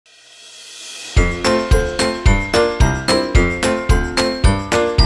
Peluches para Día del Cariño
Encuentra en Juguetón los peluches más lindos para celebrar este 14 de febrero.